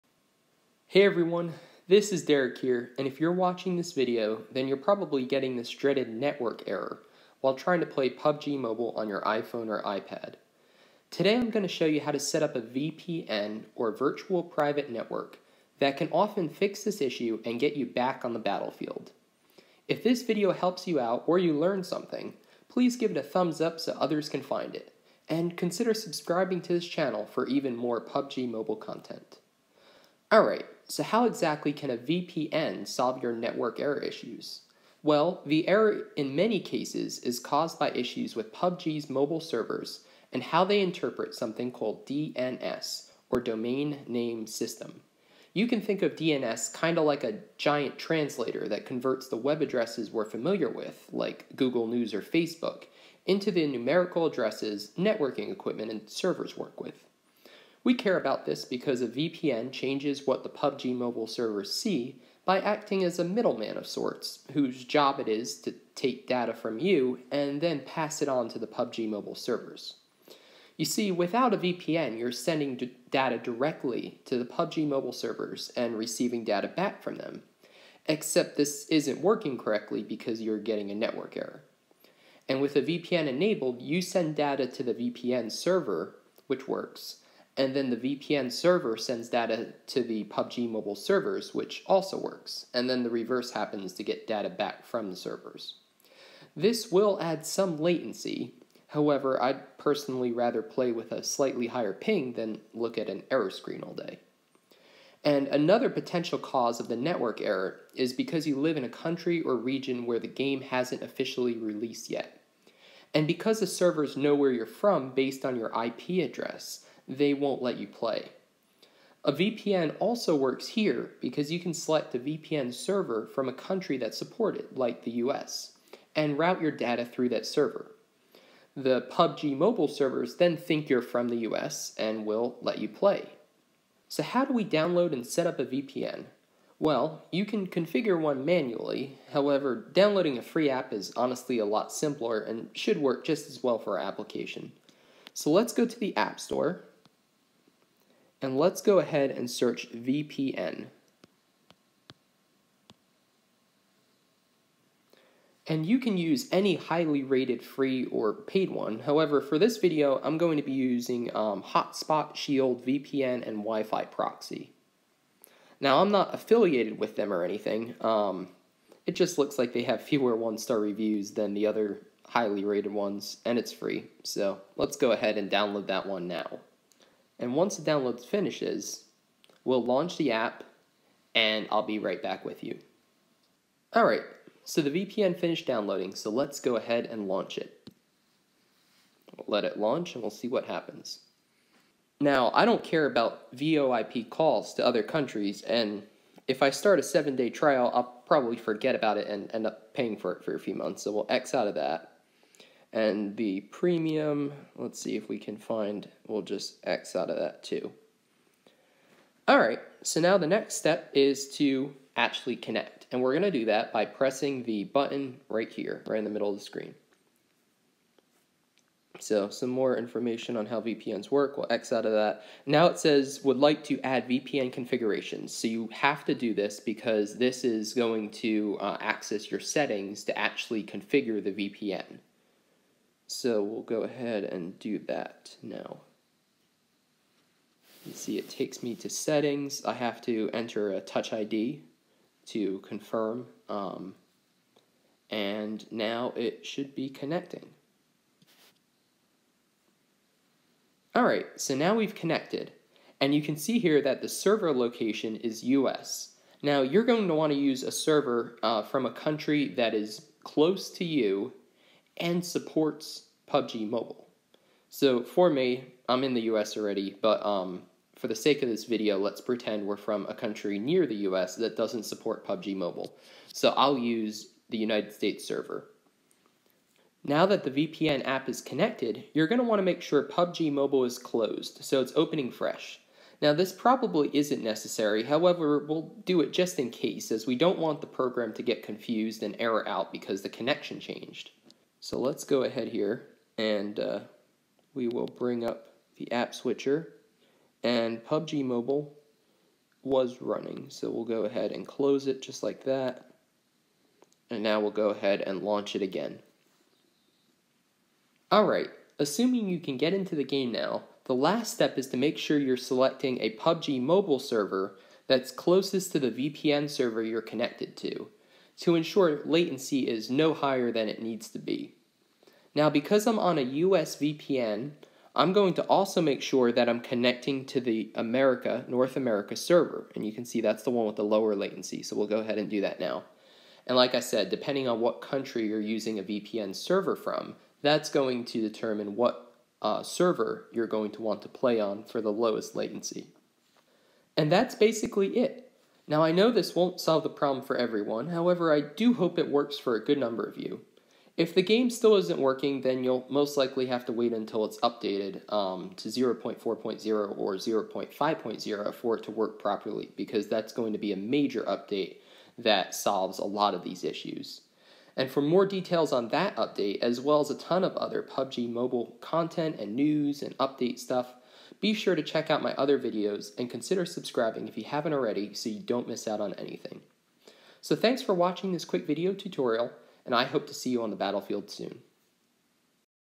PUBG Mobile - How to Fix Internet Error - V.P.N. Tutorial
Are you getting an Internet Error or Network Error when launching or trying to play PUBG Mobile? In this video I talk you through the setup of a VPN or Virtual Private Network to (hopefully) fix this issue.